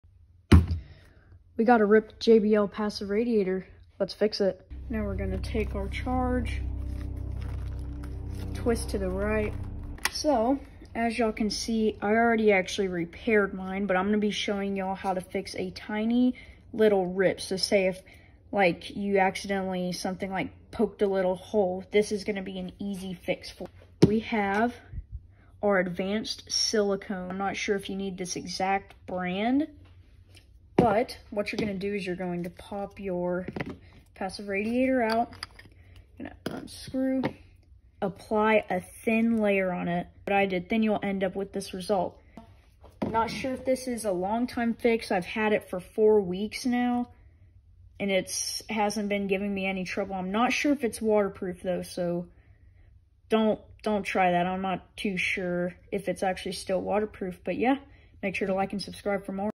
How to fix a riped/torn jbl passive radiator or any pr (easy fix) (NOT for huge tears) (quick fix)
So today I will be showing you how to fix any type of passive radiator not a huge tear but just a small tear so first thing you wanna do is get your passive radiator take the silicon glue and cake it on there let it dry up I would say give it a day and once you have that done make sure to tested to make sure there’s no air leaks the only thing you will hear from it is a little bit of clicking from the radiators moving but nothing that makes the speaker a defect if love this way to fix it make sure to sub because when I first got a hole in my radiator I thought it was done could never be fixed the speaker was never going to be fixed ever again but I was able to try this and it worked so let me know if it was HELPFULL in the comments